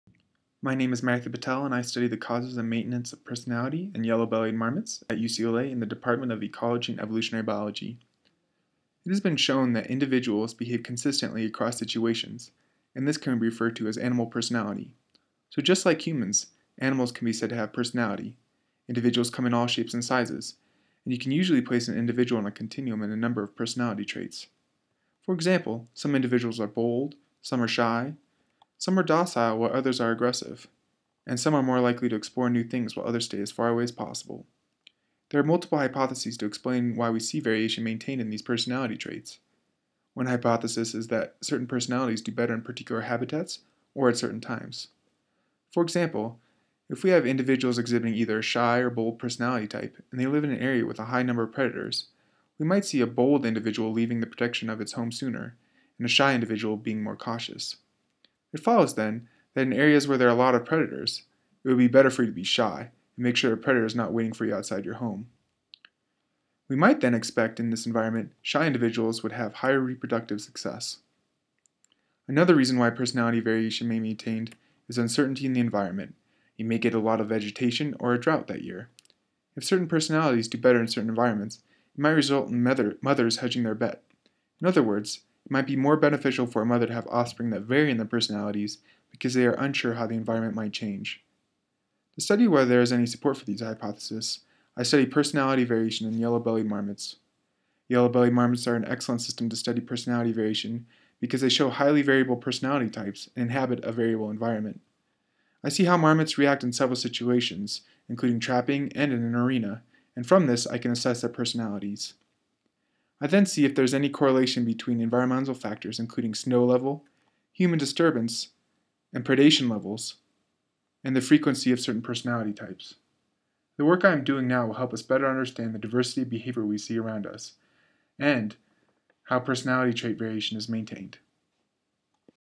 EEB Graduate Student Matthew Petelle on Yellow-Bellied Marmots
Animals, like humans, may behave in consistent ways and hence be described as having 'personality'.  Most of the research concerning animal personality has focused on proximate questions about the development and repeatability of personality types, while a fundamental understanding of the adaptive basis of personality variation has been relatively un-studied. My research focuses on the fitness consequences of personality variation, and tries to understand the ecological, social, and genetic mechanisms responsible for causing and maintaining that variation within personality types.